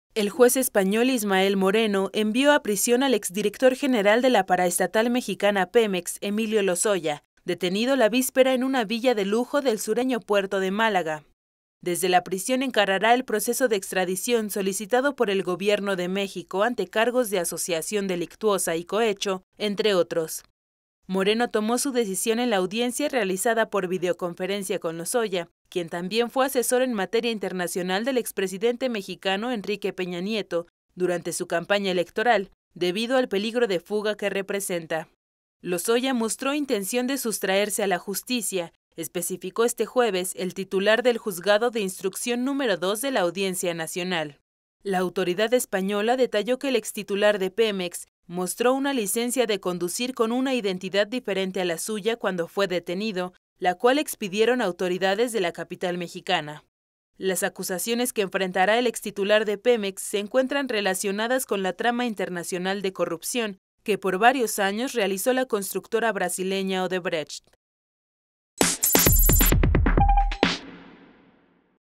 Emilio Lozoya encarará extradición en prisión, decide juez español
El juez español Ismael Moreno, envió a prisión al ex director general de la paraestatal mexicana PEMEX, Emilio Lozoya, detenido la víspera en una villa de lujo del sureño puerto de Málaga.